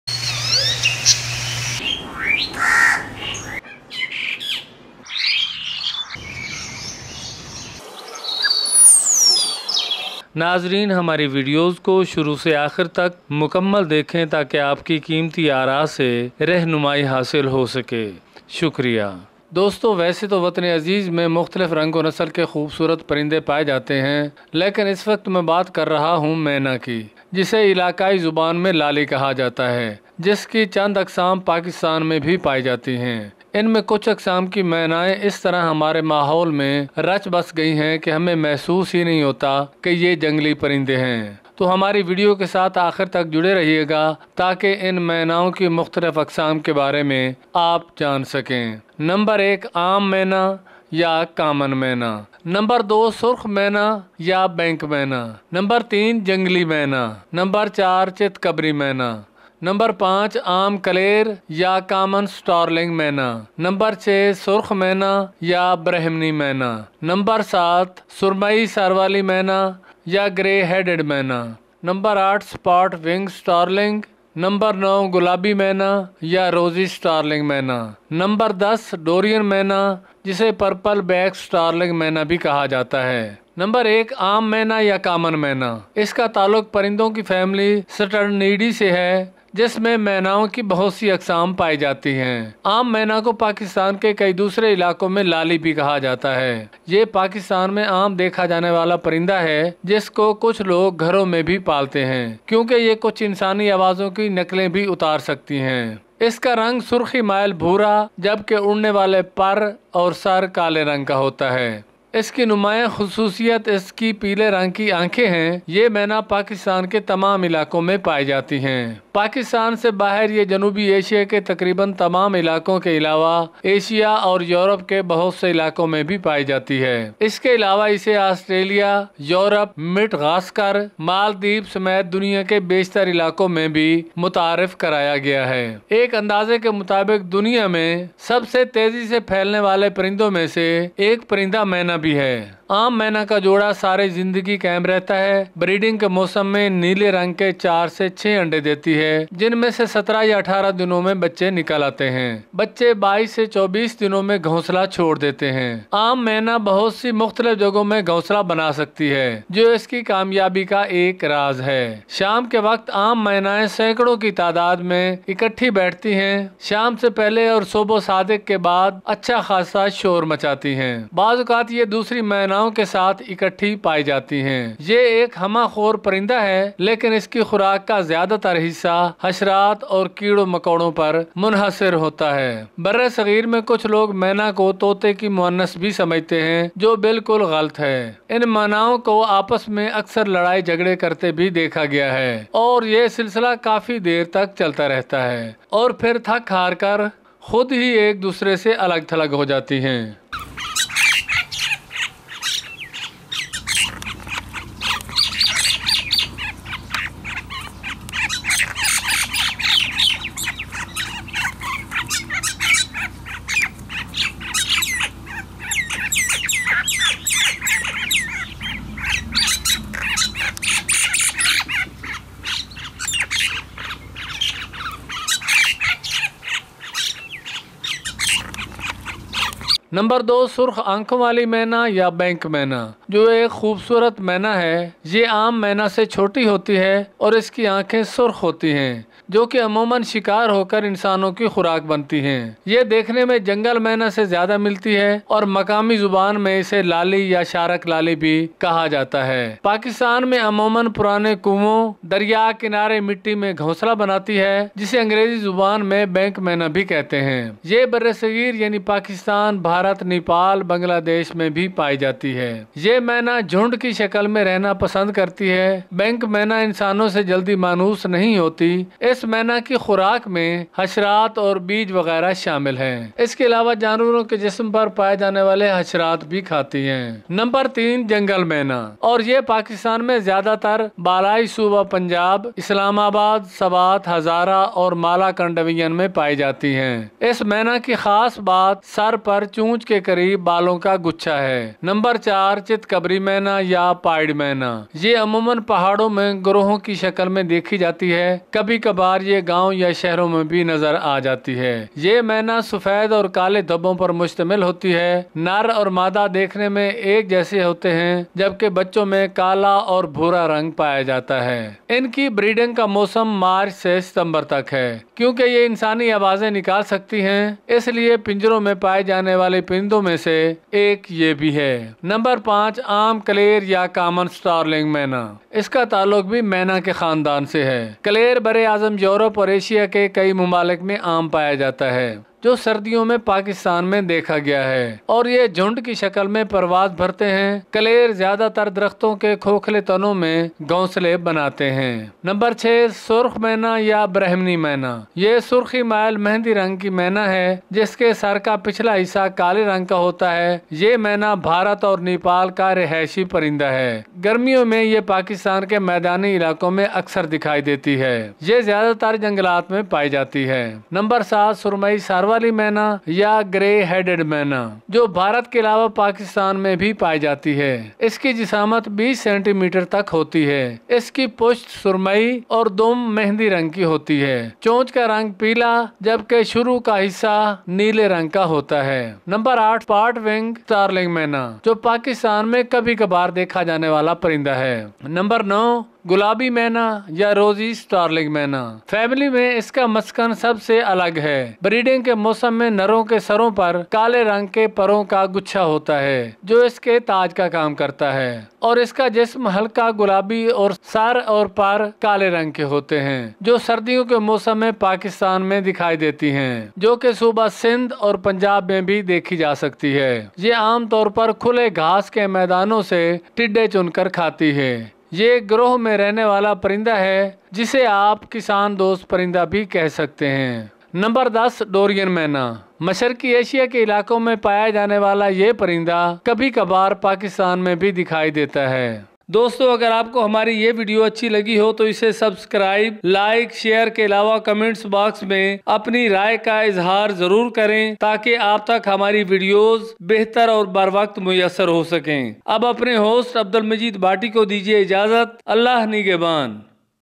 Surprising types of MYNA (مینا) | Information About Myna || wild bulbuls of pakistan || birds tips
Birds of different colors and breeds are found in the country of Pakistan, in which the Myna has a unique identity in the environment and culture of Pakistan. Myna is also known as “Lali” in common parlance. Let us talk about the Myna in detail and examine its different types and their characteristics. I hope you will like this information about Myna, which will increase your knowledge.

مینا یعنی لالی پر ایک مختصر مگر جامع تحقیقاتی ویڈیو جو کہ آپ کے علم میں بہترین اضافہ کرے گی۔